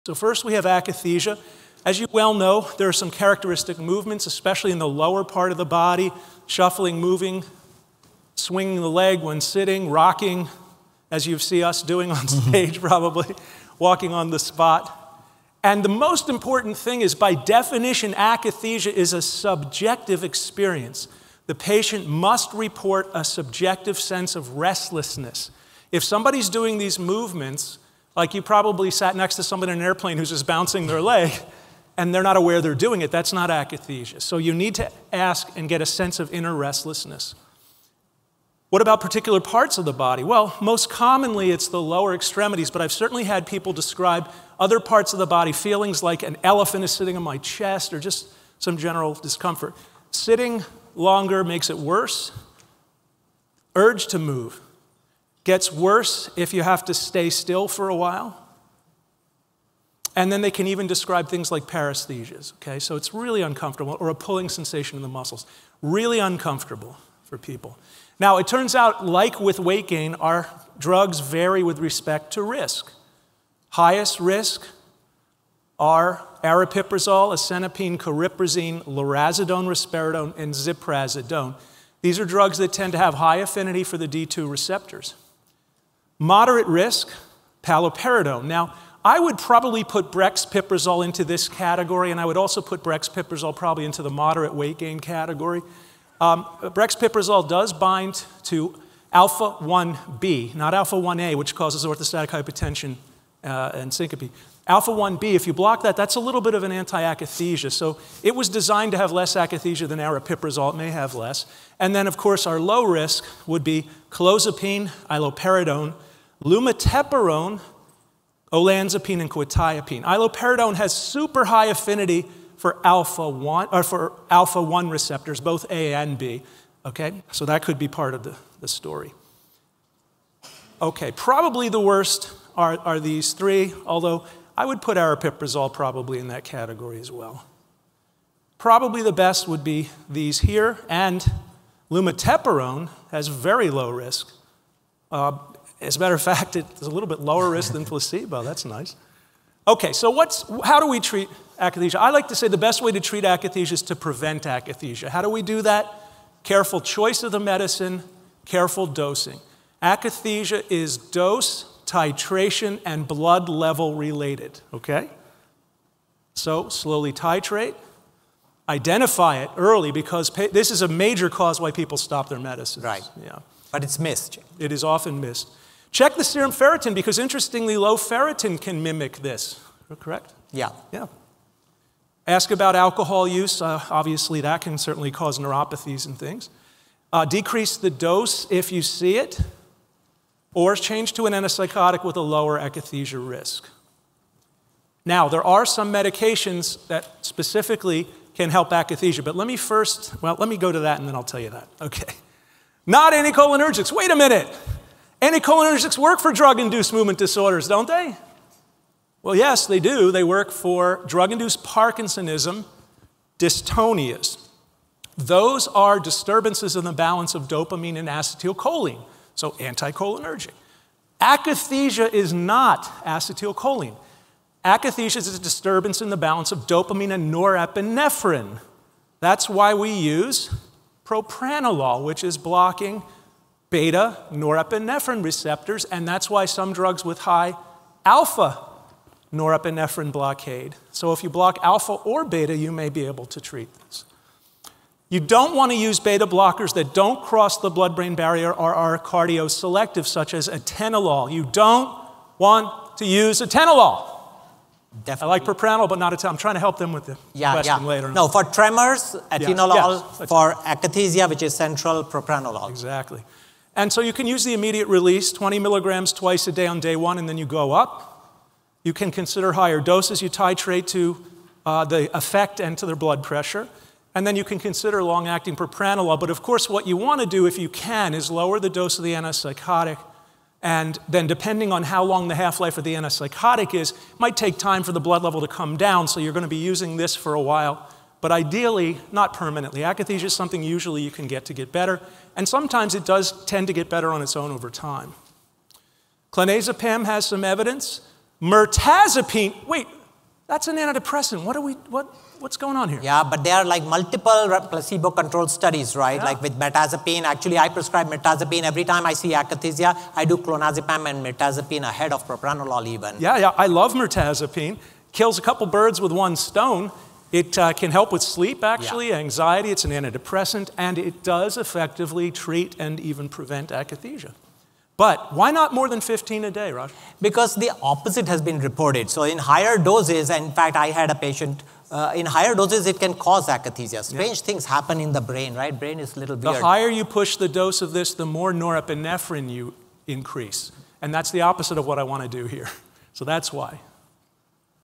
Akathisia: Prevention, Evaluation, and Management
Dr. Andrew Cutler and Dr. Rajnish Mago describe the characteristic signs and symptoms of akathisia, as well as discuss the prevention, evaluation and management of this antipsychotic side effect.

Watch the free CME/CE lecture, titled “Balancing the Scales: Antipsychotic Side Effects",

Listen to NEI Podcast!